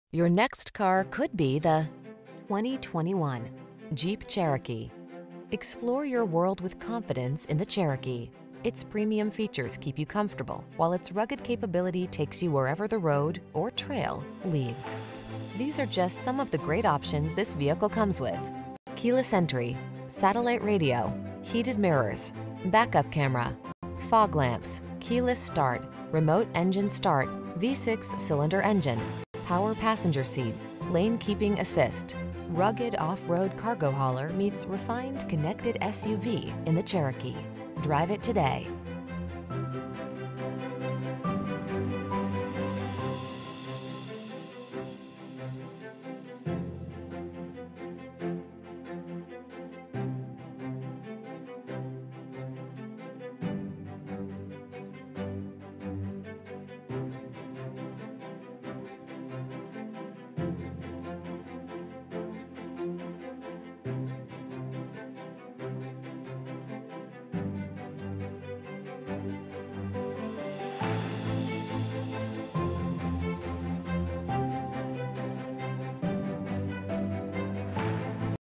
2021 Jeep Cherokee Monrovia, Pasadena, Los Angeles, Montebello, Azusa, CA B1227359
Granite Crystal Metallic Clearcoat New 2021 Jeep Cherokee available in monrovia, California at Monrovia CDJR. Servicing the Pasadena, Los Angeles, Montebello, Azusa, CA area.
2021 Jeep Cherokee Latitude Lux - Stock#: B1227359 - VIN#: 1C4PJLMX1MD227359

Monrovia CDJR
1305 S Mountain Ave